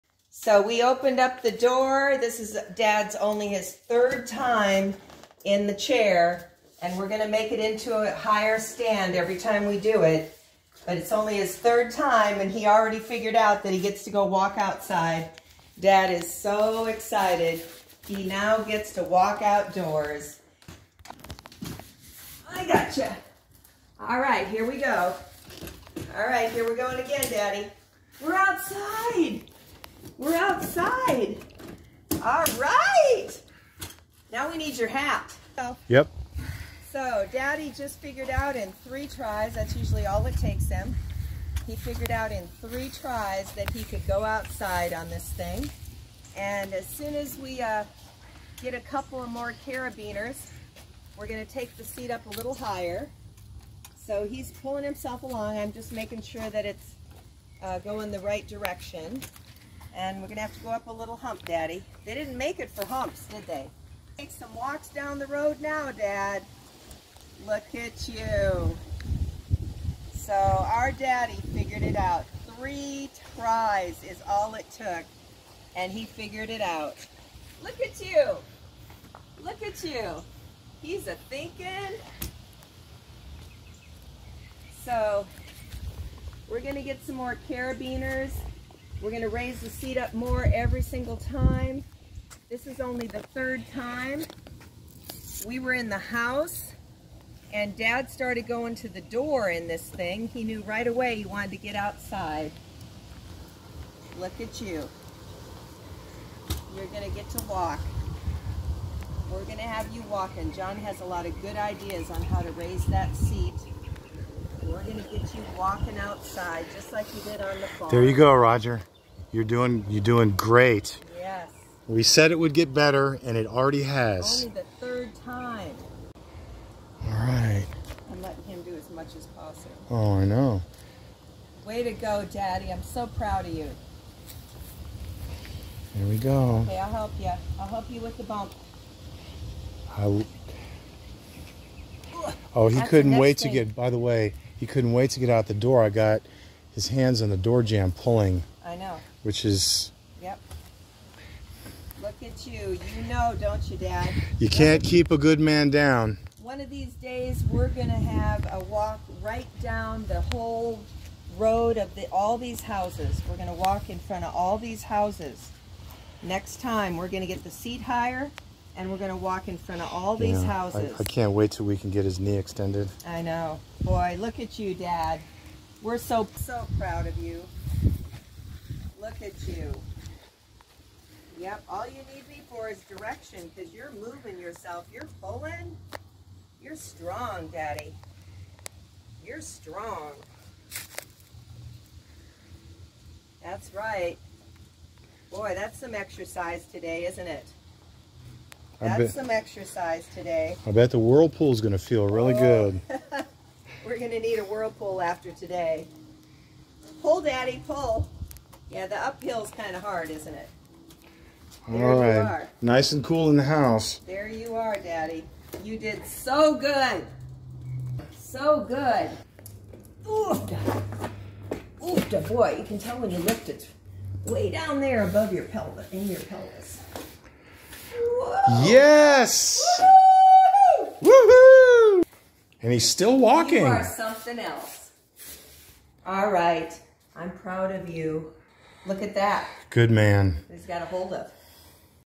(July 24, 2022) Only 3rd Time Dad wanted to go outside!
We're working on getting the seat higher so Dad can extend his leg straight.  This is only his 3rd-time in the Stand-Walker.  The 1st-Time, he wanted nothing to do with it; the 2nd-Time, he understood and began using his good leg right away.  Today, he headed for the front door right away!! John used to bike all over NYC; he's getting carabineer's to modify the seat higher -- so Dad will be able to straighten his good leg and "stand up".  It's getting so exciting ~~ only his 3rd-Time!!